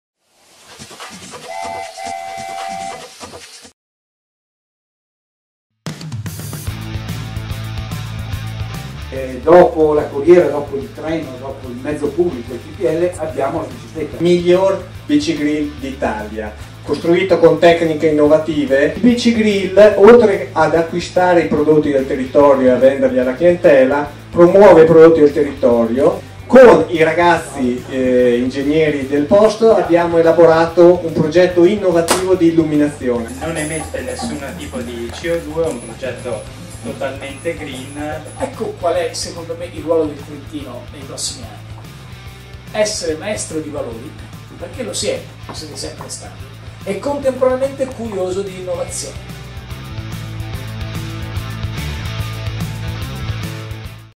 48bis - Faedo 14/11
Visita alla Bike Break Rotaliana di Faedo, una nuova organizzazione che offre ristoro e servizi a ciclisti, turisti e famiglie, lungo la pista ciclabile Valle dell'Adige.